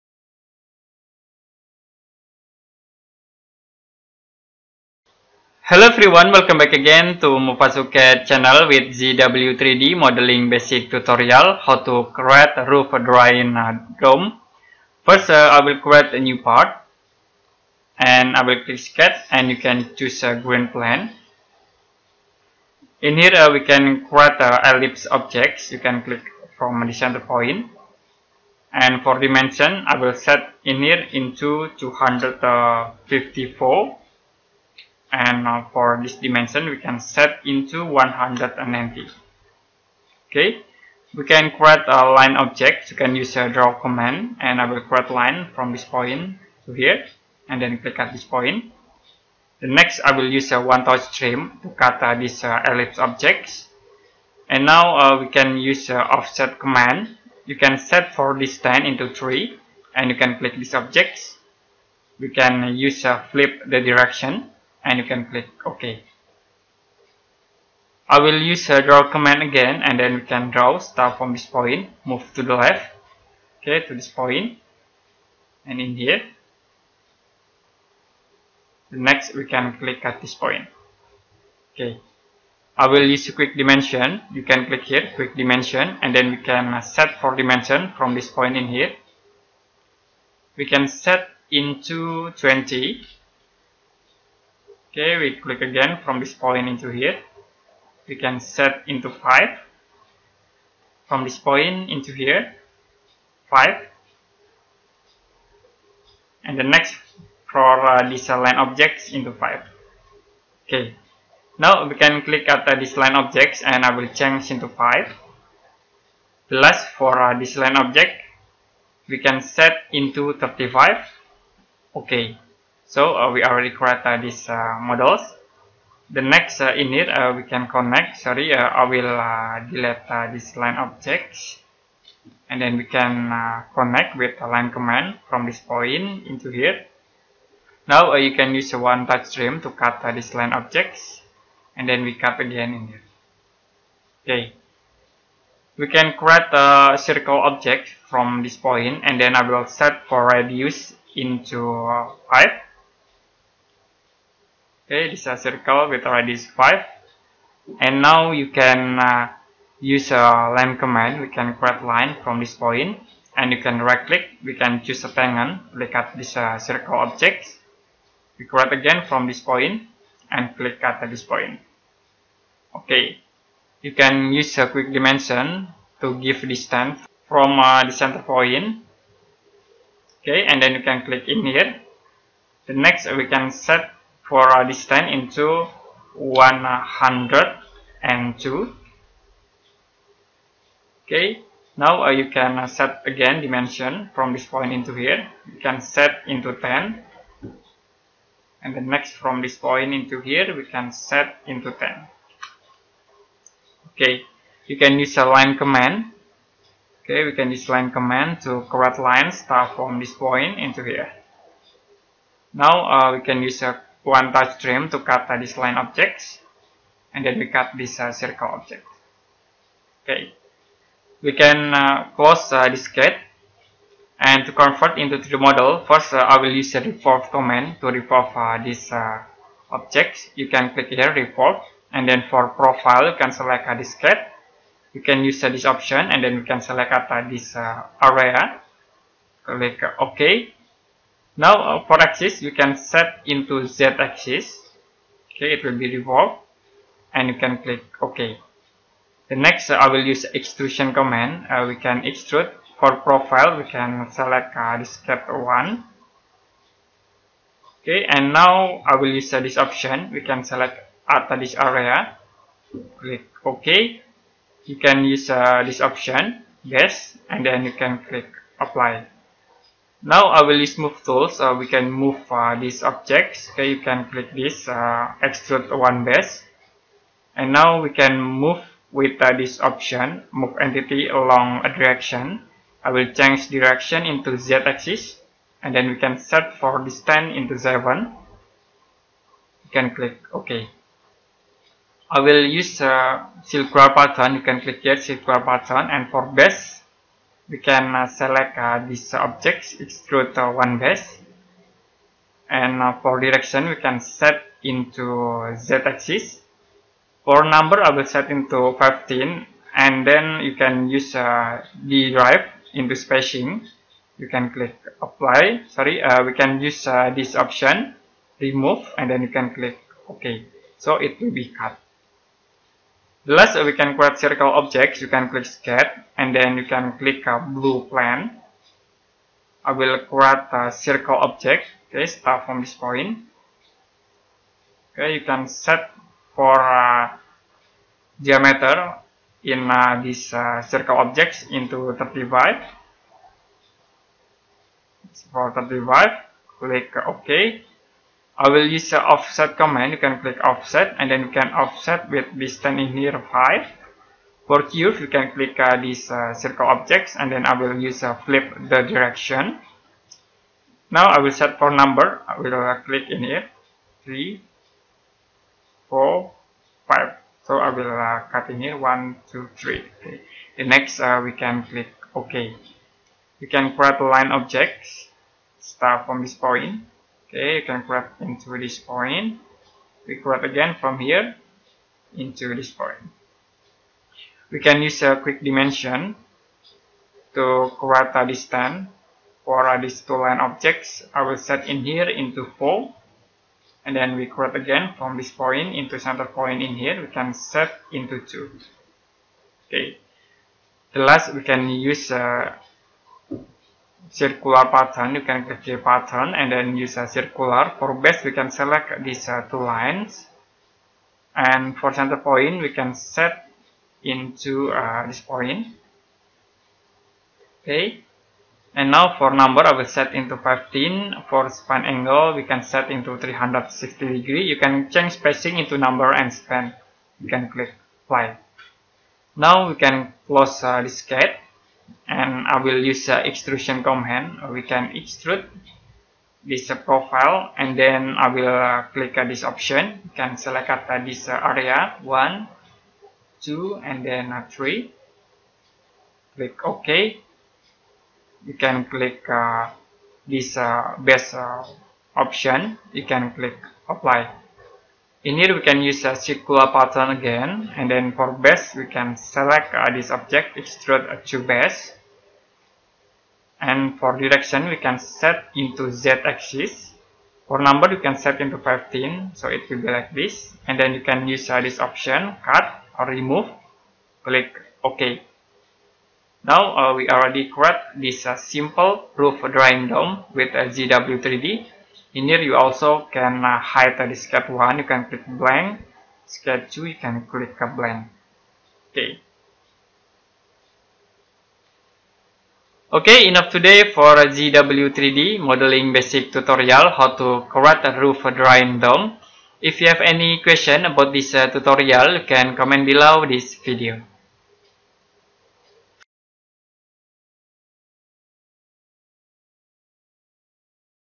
#21 ZW3D 2020 Modeling Roof Drain Dome Basic Tutorial For Beginner
Learn ZW3D how to create 3d roof drain dome with some easy command for beginner, check it out!!!